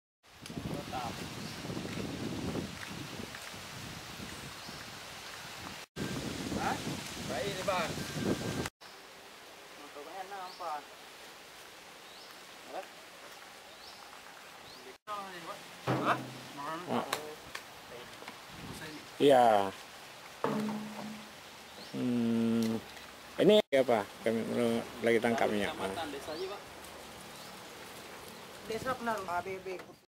SPBU di Aceh Timur Jual BBM Pakai Drum
Metrotvnews.com, Aceh Timur: SPBU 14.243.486 yang berada di Jalan Medan-Banda Aceh, Desa Seuneubok Teungoh, Kecamatan Peudawa, Aceh Timur, Kamis (24/4/2014), menjual BBM ke masyarakat dalam drum dan jerigen. Praktek yang dilakukan hampir setiap hari ini jelas melanggar undang-undang nomor 22 tahun 2001 tentang minyak dan gas bumi, selain itu juga membuat stok BBM sering kosong.